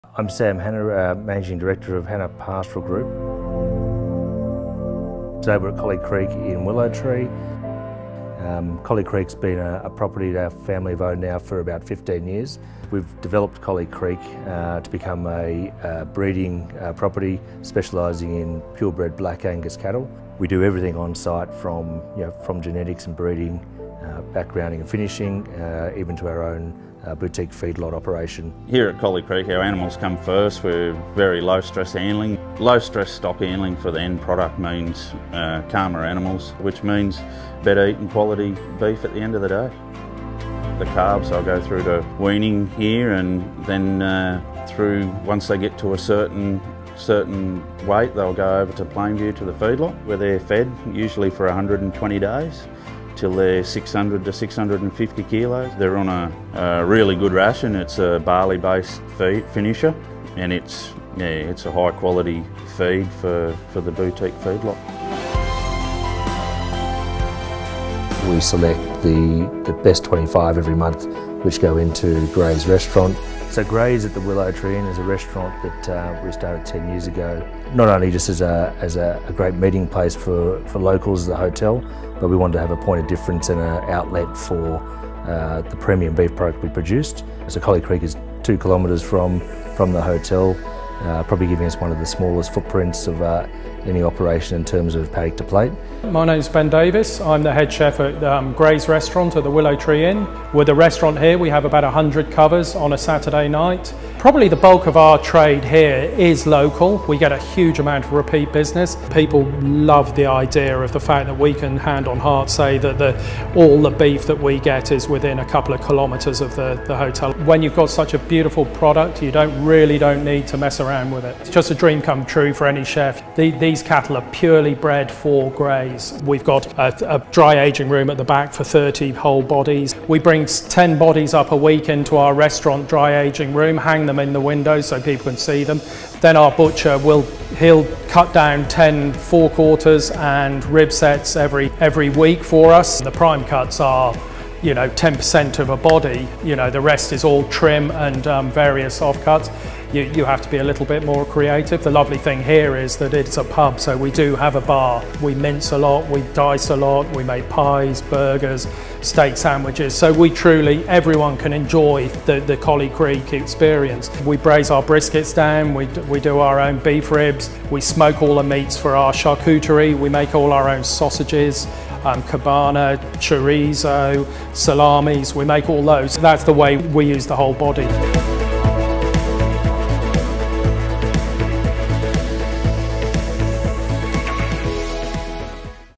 Paddock to Plate: Graze at the Willow Tree Inn
We visit the team at Graze in the NSW country town of Willow Tree - a paddock to plate destination producing, processing, ageing, butchering and serving their own Colly Creek Angus beef.